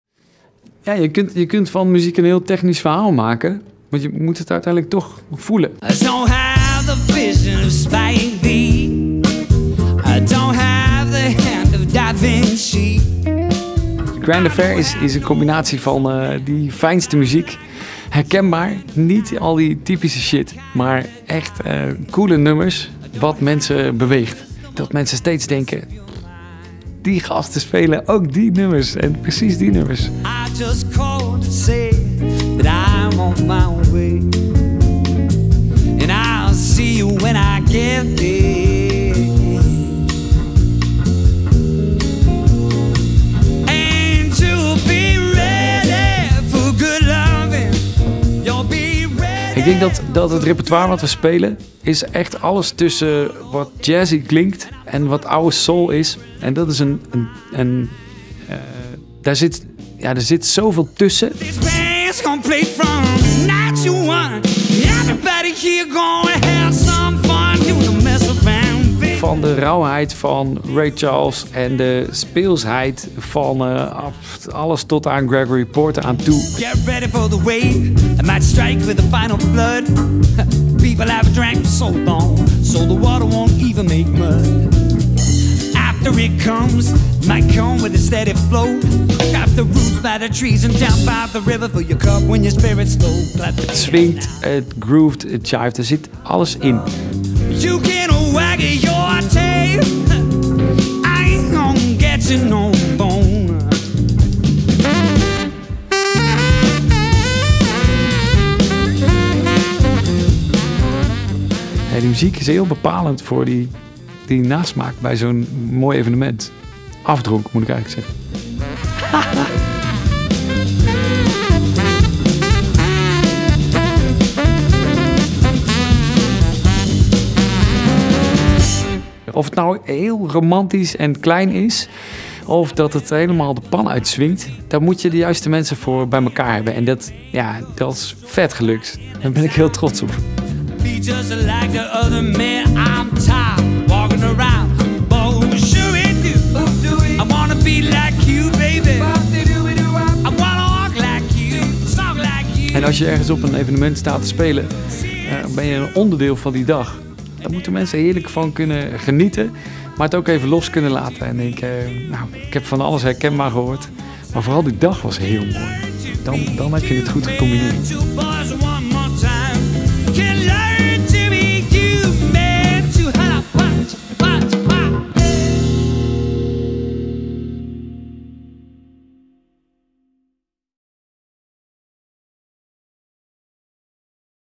The Grand Affair - Official 2024
'The Grand Affair' is a well sought after music ensemble from the Netherlands. This film was recorded by the amazing Jan Persoon @ City Lounge - Apeldoorn.

Benjamin Kuijpers | leadvocals & saxophone
Job Kaihatu | guitar & backing vocals
Maarten Bakker | upright bass & backing vocals
Tim Beudel | drums & backing vocals

Recording & Production : NLS Music
Microphones : Gonzo Audio
Location / Studio : CityLounge Apeldoorn
Video Film & Production : Jan Persoon Videografie